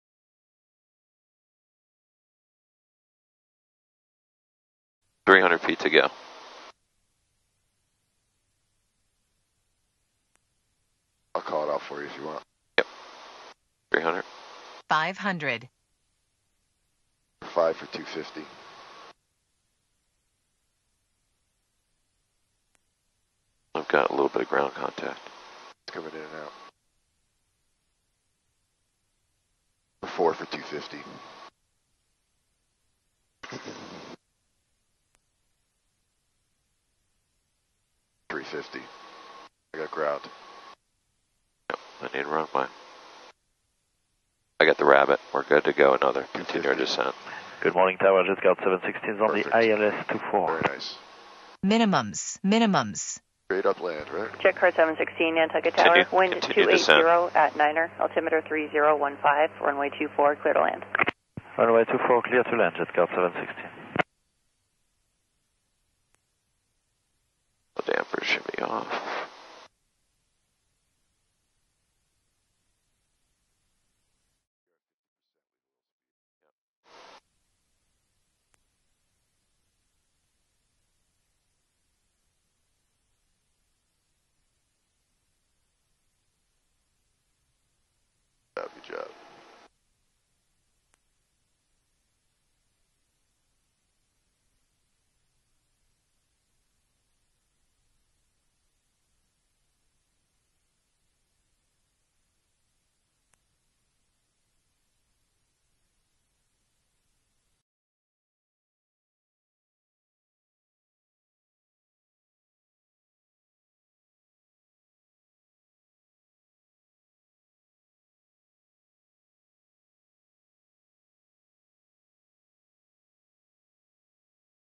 Cirrus Flight 2019; ACK (Nantucket) to Minimums
I have flown into ACK dozens of times and it seems that the fog layer almost always sits 50 feet above the RNAV or ILS mins.
Enjoy this quick trip and my first published video.
Thanks

Louis
The Deliman